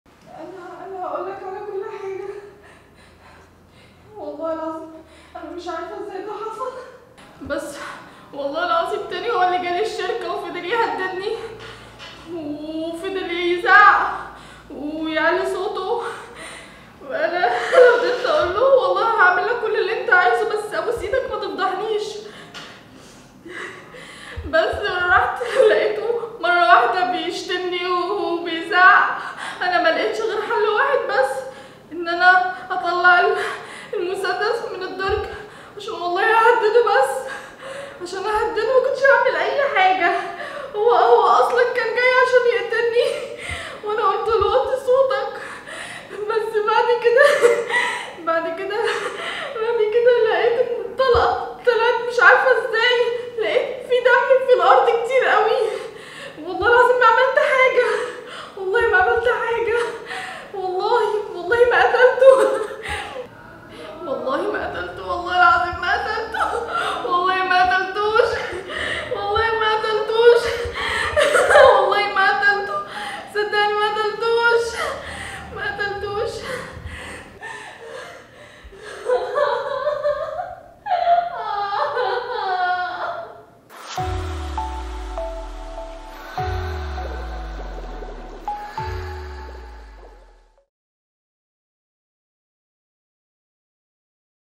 مشهد المتهمة اداء خريجي ورشة التمثيل في ترند ميديا اكاديمي
للاستفسار عن جميع الورش في مجالات التمثيل و الإخراج و السيناريو مع اكبر النجوم
للاشتراك في فرع الإمارات  يرجي الاتصال علي رقم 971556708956+ واتس اب او فون ،
للاستفسار و للاشتراك في فرع مصر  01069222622 واتس اب او فون